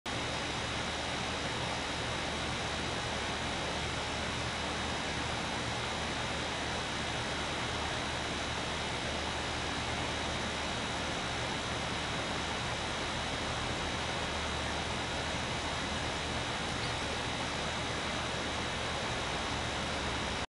Brave Little Sparrow is thirsty.
A pretty little sparrow needs a drink.

House Sparrow (Passer domesticus)